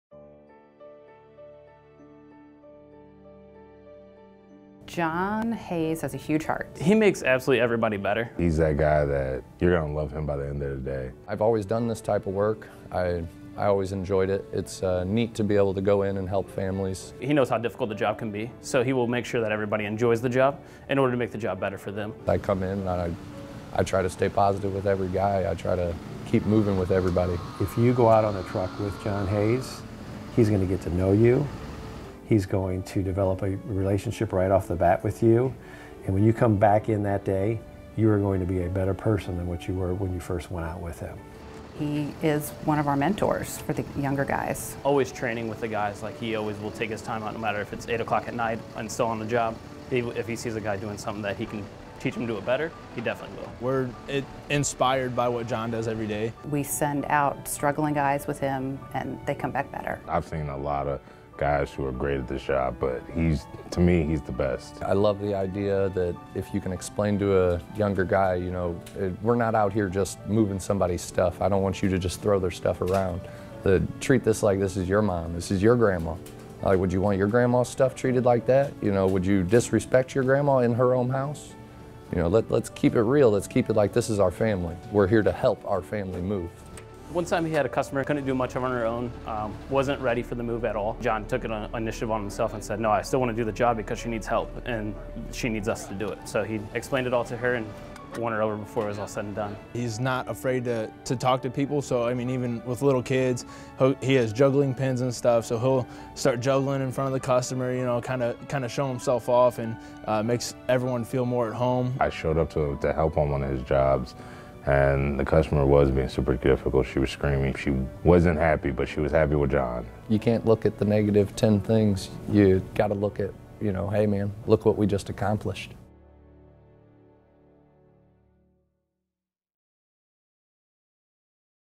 John Hayes - TWO MEN AND A TRUCK® 2019 Move Hero
John Hayes from St. Louis, Misourri, was named a runner-up for the TWO MEN AND A TRUCK® 2019 National Move Hero.